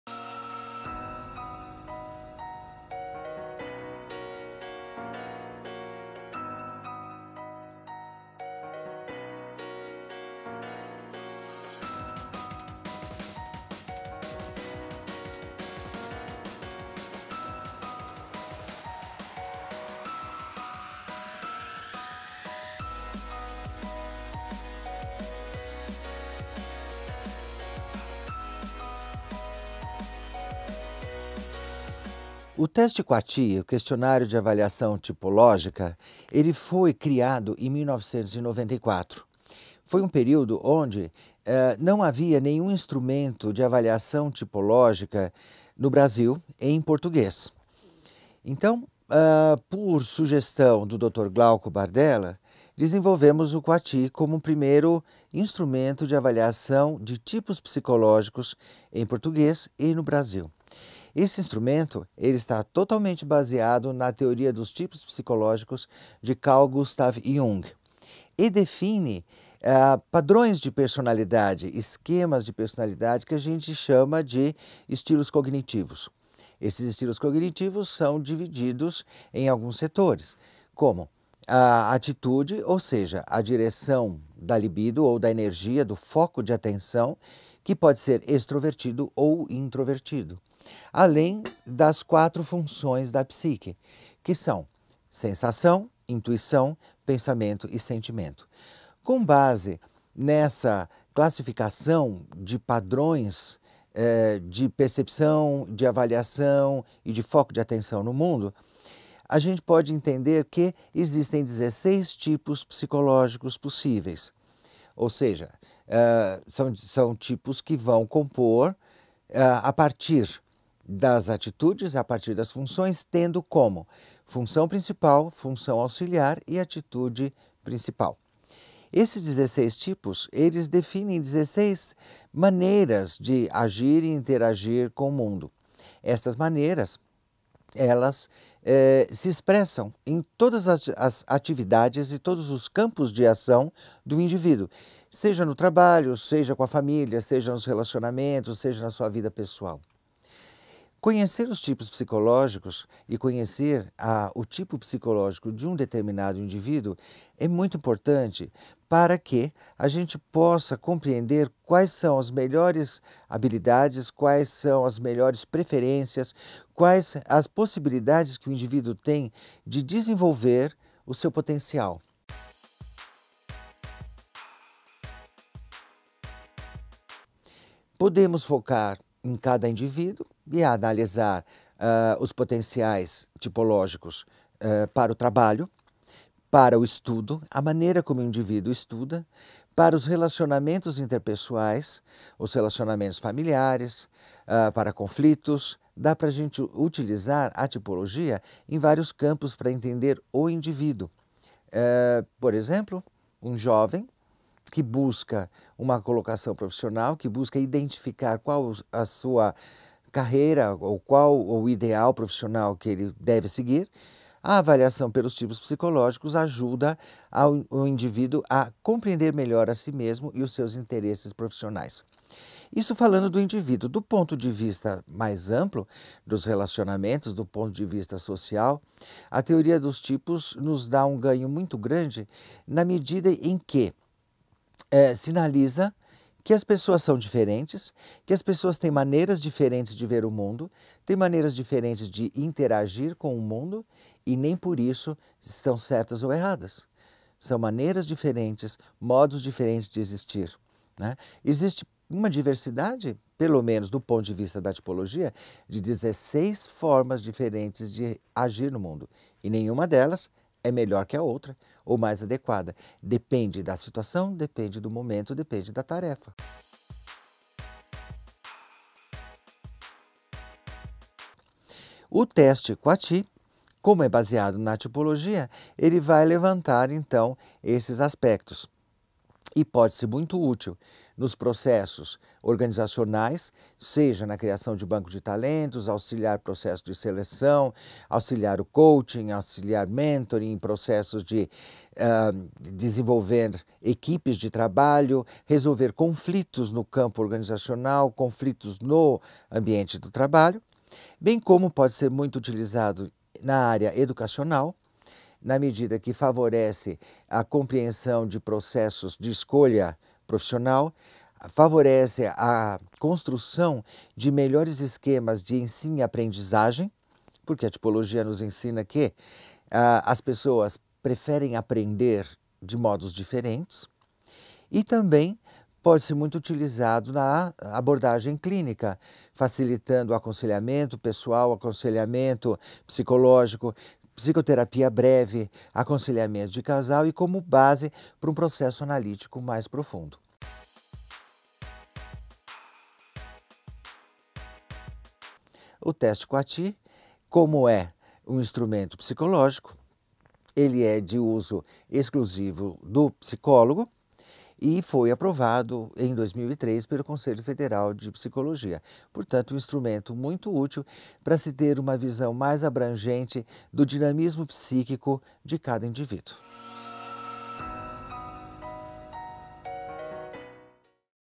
Vetor Editora Entrevista - José Jorge de Morais Zacharias | Teste QUATI
O Doutor José Jorge de Morais Zacharias fala sobre o teste Quati, um questionário baseado nos tipos psicológicos de Jung, composto por 6 propostas de situações cotidianas, cujo objetivo é avaliar a personalidade por meio de escolhas situacionais.